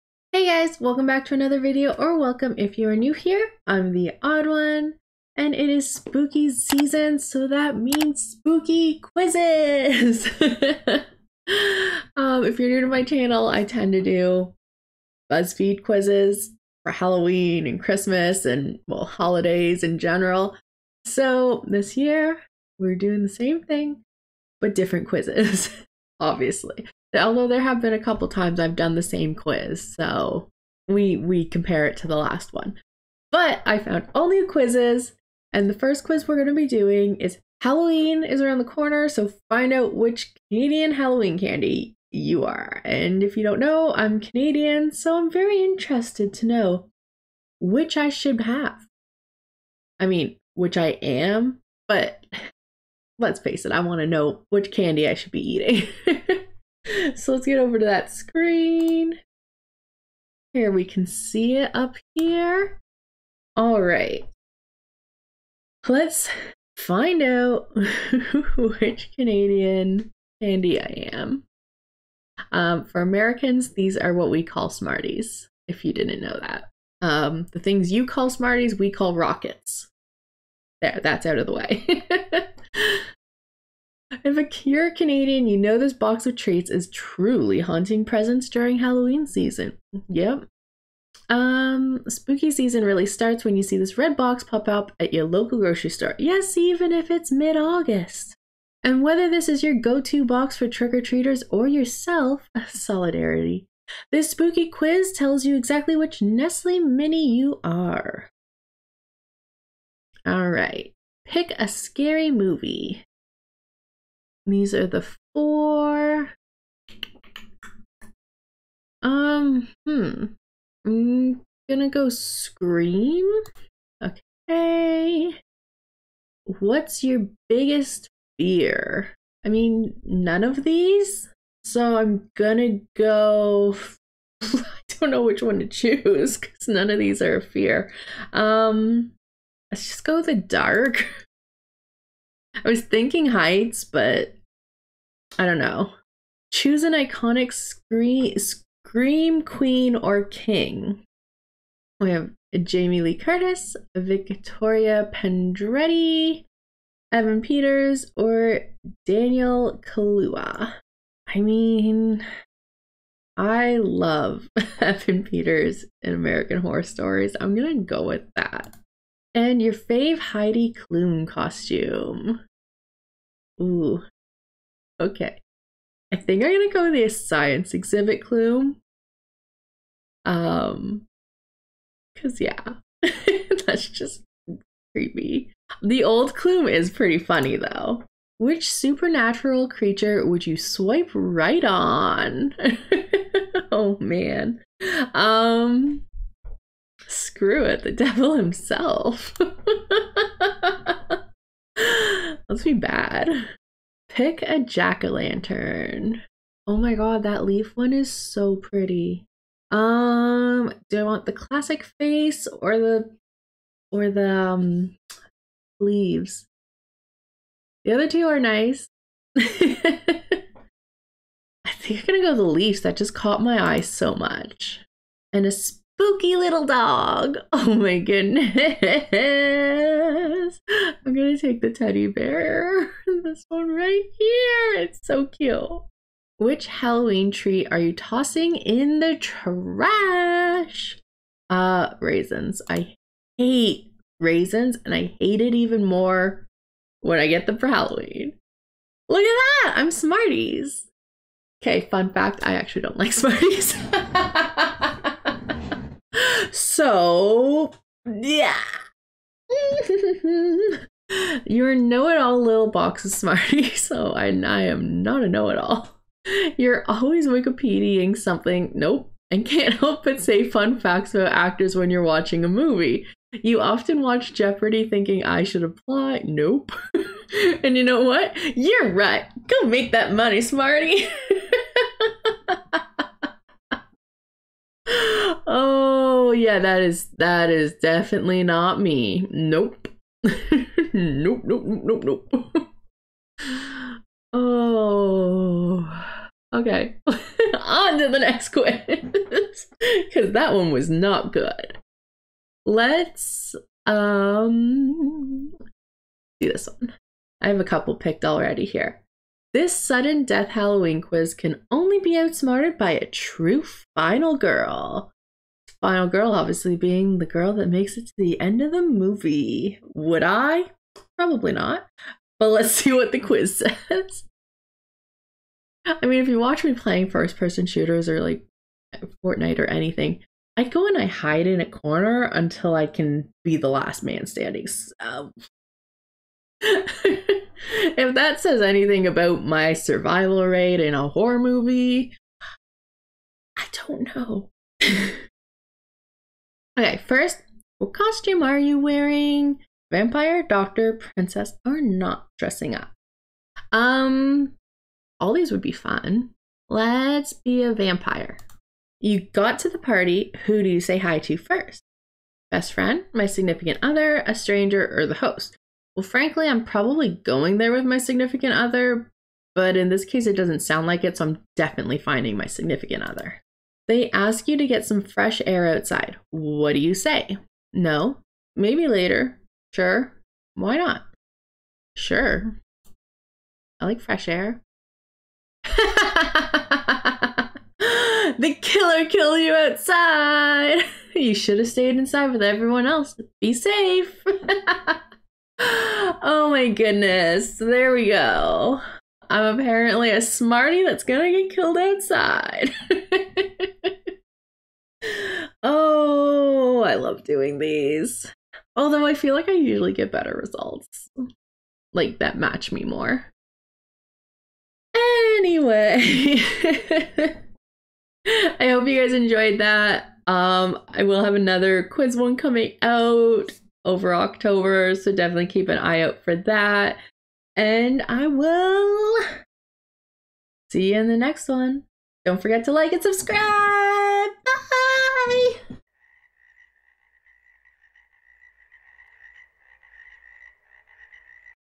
finding out which canadian halloween candy i am
Welcome back guys!!! It's our first halloween quiz of the season!